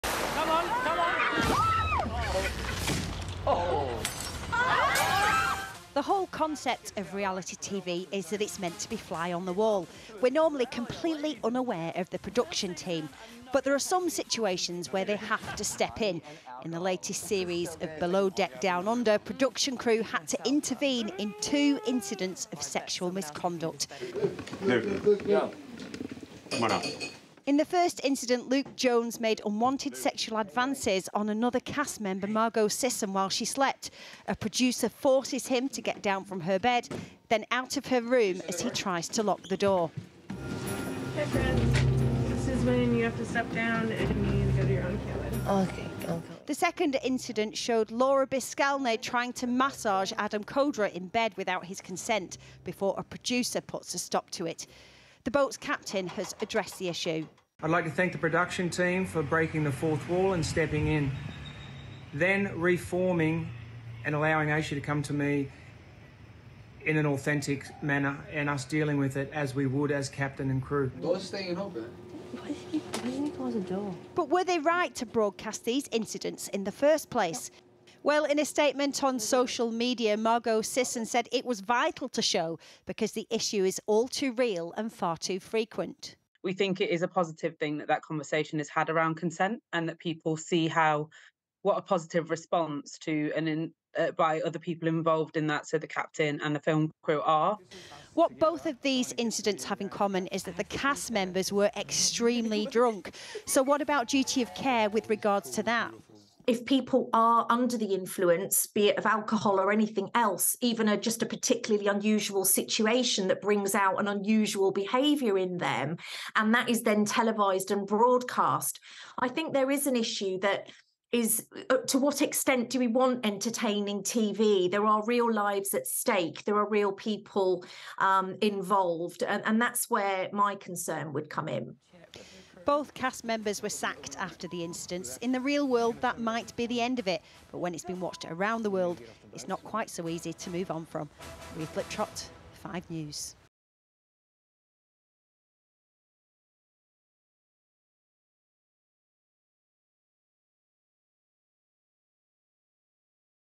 Reality TV 'Below Deck Down Under' crew address scandal | 5 News
►Two cast members from the Australian reality tv series 'Below Deck Down Under' have been sacked amid a sexual misconduct scandal on the show's set

► Luke Jones and Laura Bileskalne were fired after separately getting into the beds of other members of the cast without consent